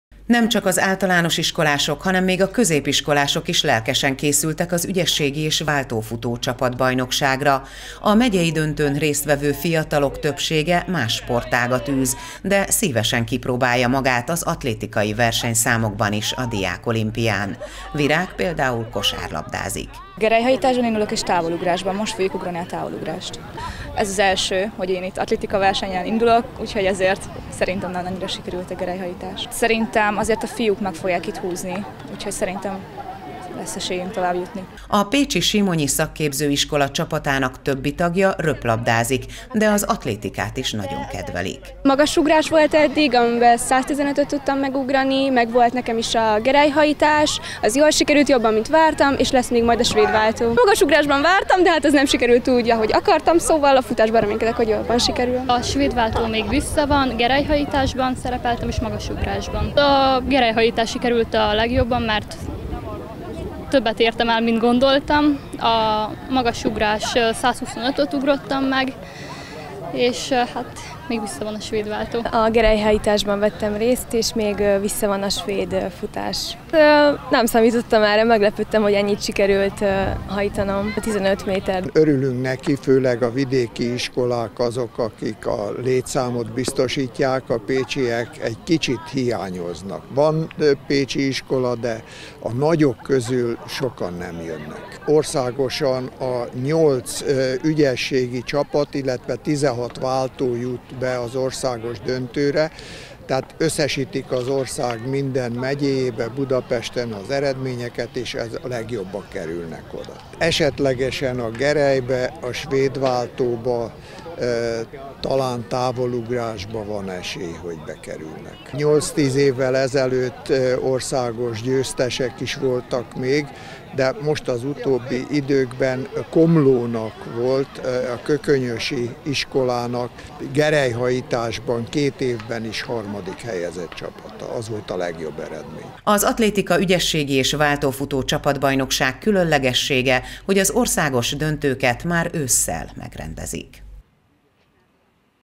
Megkezdődött az őszi atlétikai diákolimpiai szezon
Az ügyességi és váltófutó csapatbajnokság megyei döntőivel megkezdődtek az őszi atlétikai küzdelmek. Először a fiatalabbak, vagyis az általános iskolások versenyeztek, másnap pedig az 5-6-dik korcsoportosoké volt a pálya. Az ügyességi csapatbajnokság különlegessége, hogy az országos döntőket már ősszel megrendezik.